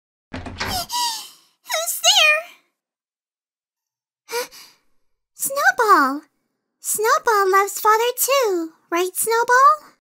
[Mad Father] Snowball Loves Father Too! [CLIP]
A small scene from my Mad Father film...

Snowball loves father too! Right? Right???

CREDITS: MacaronParisPretty, tehrainbowllama, scarletrose101

CAST:
Dolly Strawberry as Aya